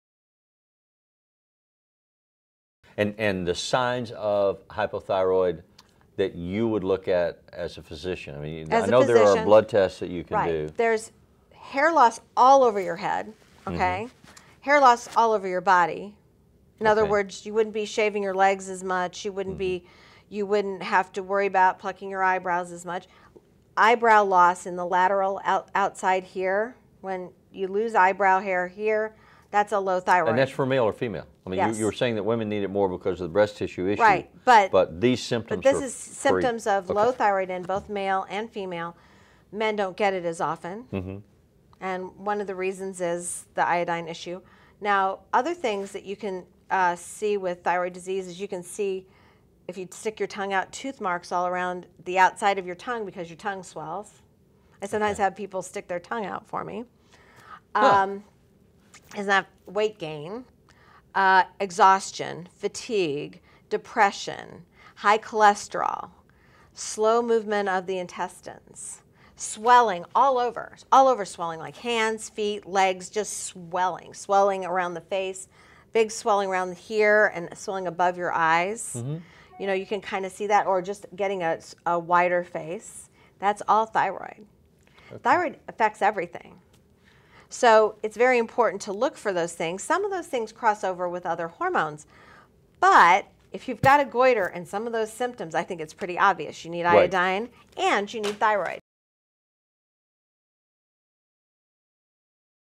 The Signs of Thyroid Disease
Signs of thyroid disease include hair loss, swollen tongue, weight gain, exhaustion, fatigue and high cholesterol. If you notice some or all of these, talk to your physician about the possibility of thyroid disease.